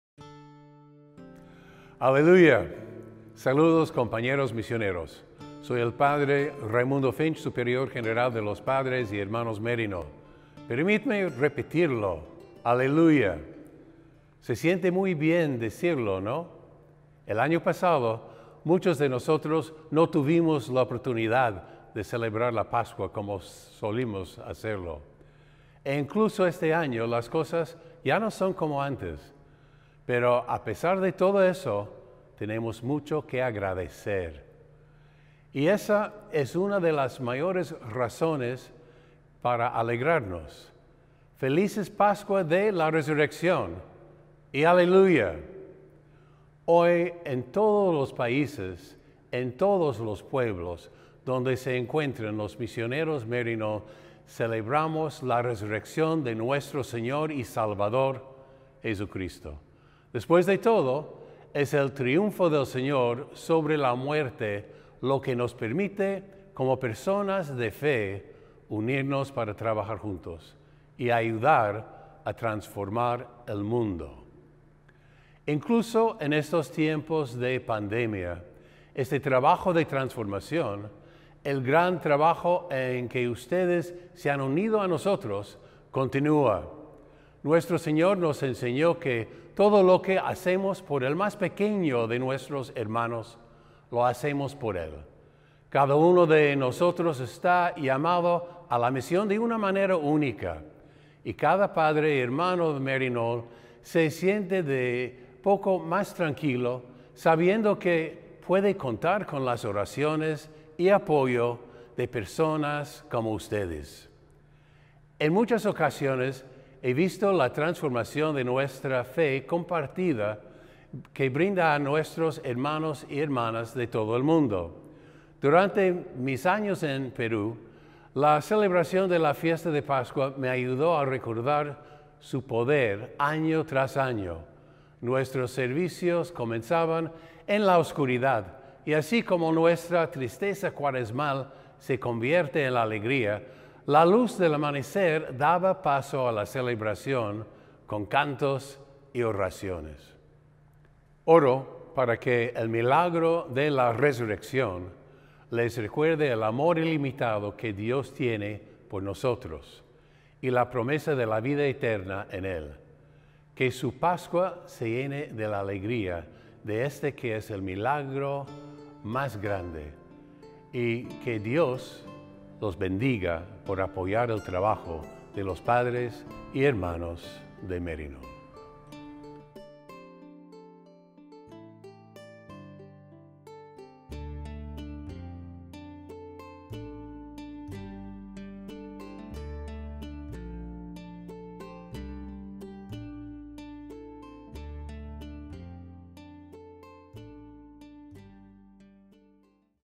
Feliz Pascua de Resurrección 2021, Padre Raymundo Finch, M.M.
¡Aleluya!

Saludos, compañeros misioneros. Soy el Padre Raymundo Finch, Superior General de los Padres y Hermanos Maryknoll. Permítanme repetirlo. ¡Aleluya!
Se siente muy bien decirlo, ¿no? El año pasado muchos de nosotros no tuvimos la oportunidad de celebrar la Pascua como solemos hacerlo. E incluso este año, las cosas ya no son como antes. Pero a pesar de todo eso, tenemos mucho que agradecer.

Y esa es una de las mayores razones para alegrarnos. ¡Feliz Pascua de Resurrección! ¡Y Aleluya!

Hoy, en todos los países y en todos los pueblos donde se encuentren, los Misioneros Maryknoll celebramos la Resurrección de nuestro Señor y Salvador, Jesucristo. Después de todo, es el triunfo del Señor sobre la muerte lo que nos permite, como personas de fe, unirnos para trabajar juntos y ayudar a transformar el mundo, juntos.

Incluso en estos tiempos de pandemia, este trabajo de transformación, el gran trabajo en el que ustedes se han unido a nosotros, continúa. Nuestro Señor nos enseñó que todo lo que hacemos por el más pequeño de nuestros hermanos, lo hacemos por Él. Cada uno de nosotros está llamado a la misión de una manera única. Y cada padre y hermano Maryknoll se siente un poco más tranquilo sabiendo que puede contar con las oraciones y el apoyo de personas como ustedes en casa.
En muchas ocasiones he visto la transformación que nuestra fe compartida brinda a nuestros hermanos y hermanas de todo el mundo. Durante mis años en Perú, la celebración de la Fiesta de Pascua me ayudó a recordar su poder año tras año. Nuestros servicios comenzaban en la oscuridad y, así como nuestra tristeza cuaresmal se convierte en alegría, la luz del amanecer daba paso a la celebración con cantos y oraciones.

Oro para que el milagro de la Resurrección les recuerde el amor ilimitado que Dios tiene por nosotros y la promesa de la vida eterna en Él. Que su Pascua se llene de la alegría de este que es el más grande de los milagros. Y que Dios los bendiga por apoyar el trabajo de los Padres y Hermanos Maryknoll.